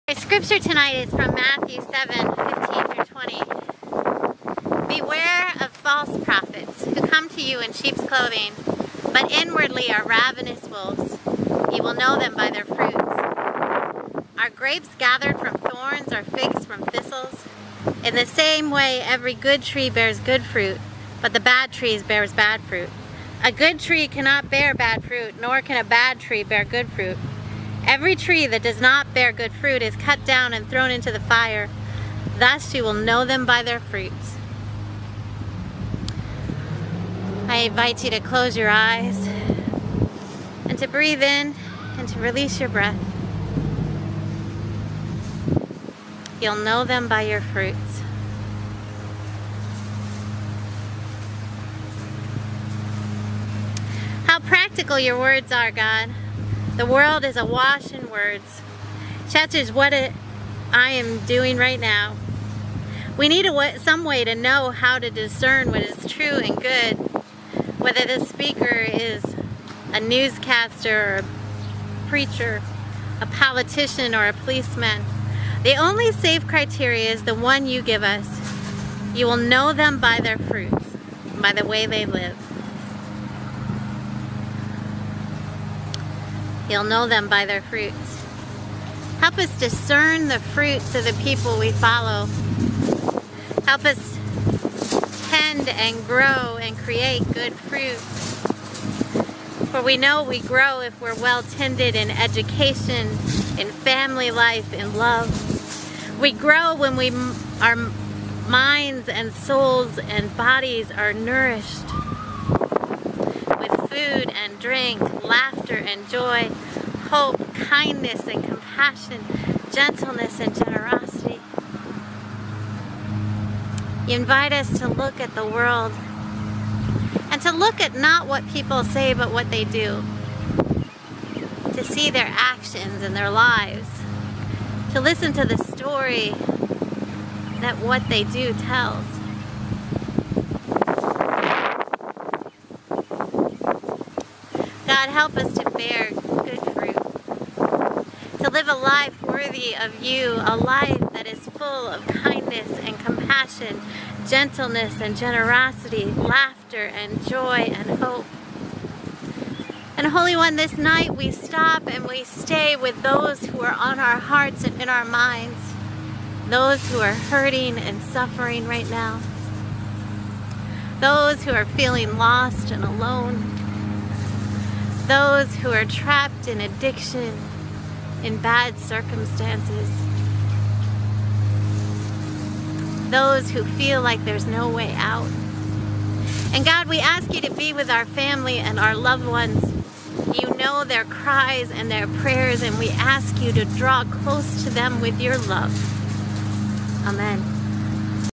How do tell if someone is being truthful? a prayer
We are approaching the end of our prayer journey through the Sermon on the Mount. This prayer is based on Matthew 7:15-20.